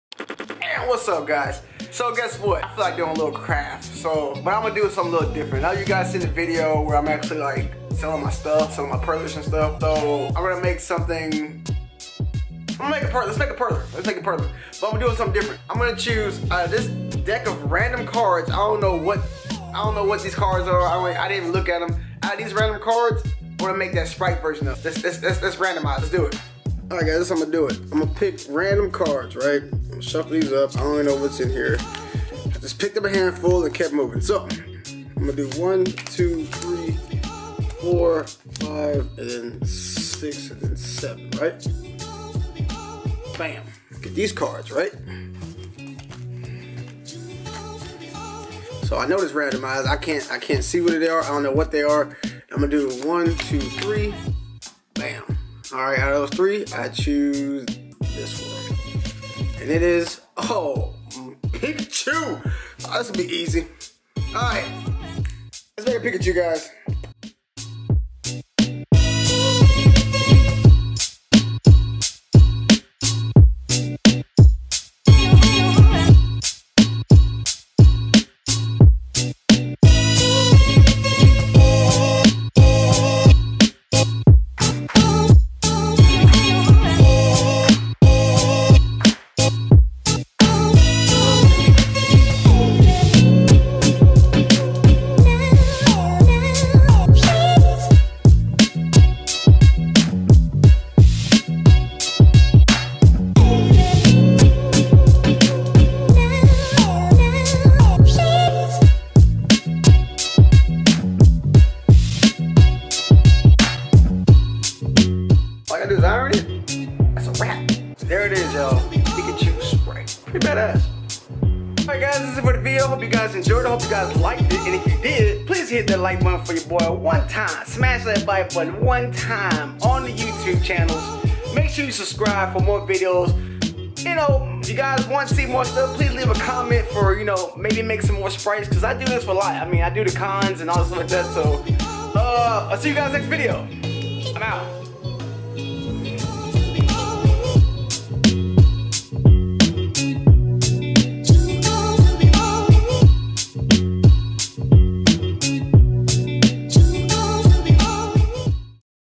LETS MAKE A PIKACHU PERLER SPRITE VLOG
I make a random sprite from randomizing the pokemon cards i got the other day. Total random so it works to my favor as you will see.